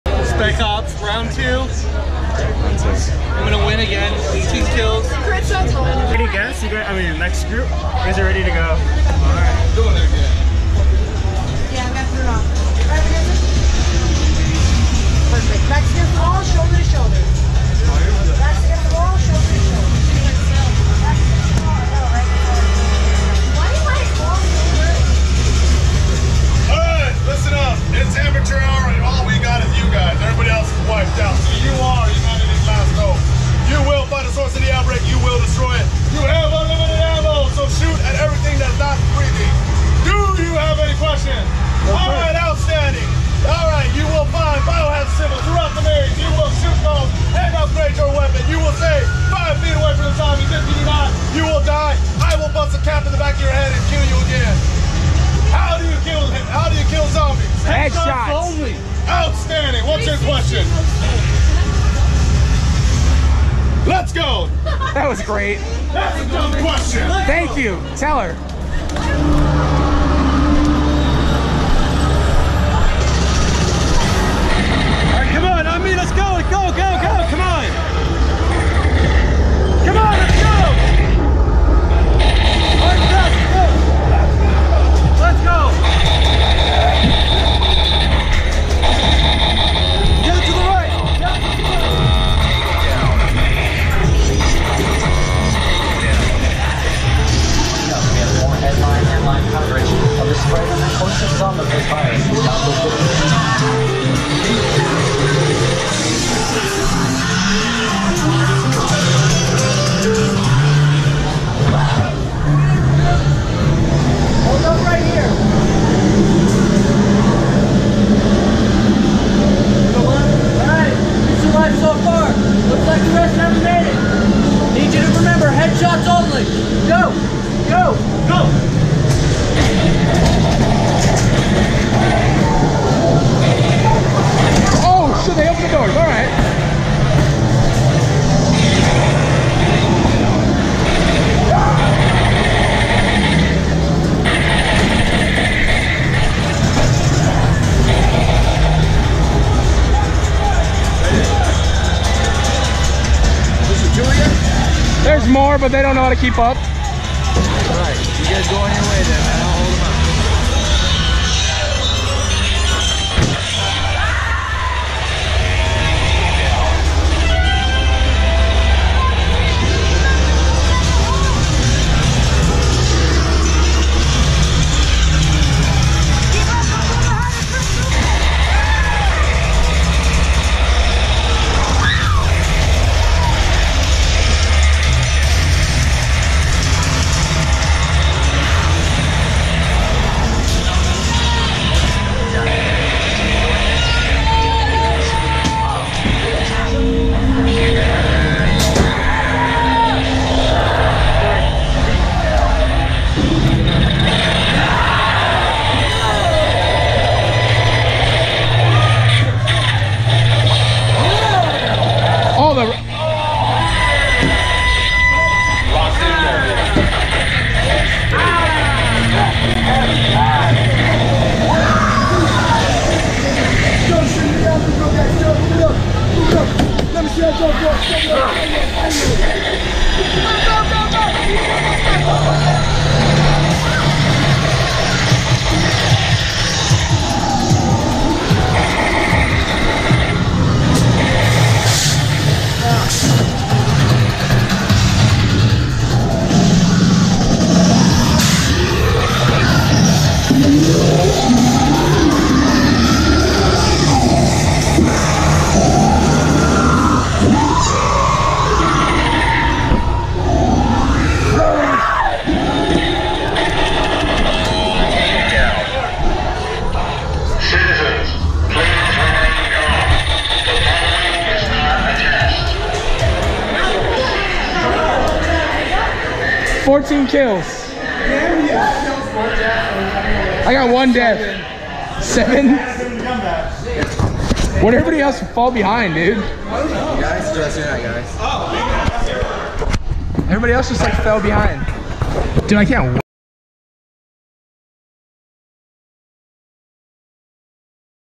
Special Ops Infected Low Light POV Walkthrough Knotts Scary Farm 2019
We walkthrough Special Ops Infected with the NOH crew!
Shoutout to Robert Lopez for the use of the Go Pro!
Other Channel's I'm involved with:
Theme Park Pass: